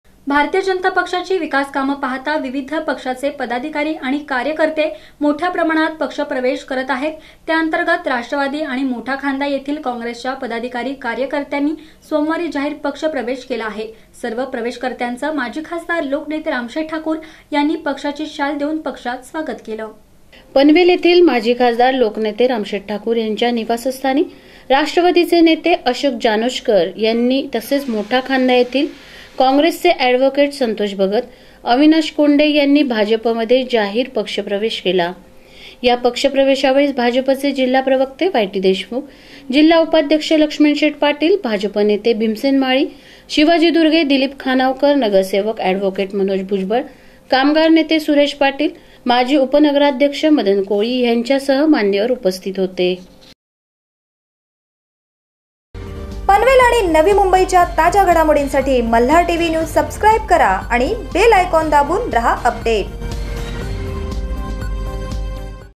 राष्ट्रवादीचे नेते अशोक जानोशकर भाजपात दाखल; लोकनेते रामशेठ ठाकूर यांच्या उपस्थितीत प्रवेश !
राष्ट्रवादीचे नेते अशोक जानोशकर भाजपात दाखल;
माजी खासदार लोकनेते रामशेठ ठाकूर यांच्या उपस्थितीत प्रवेश !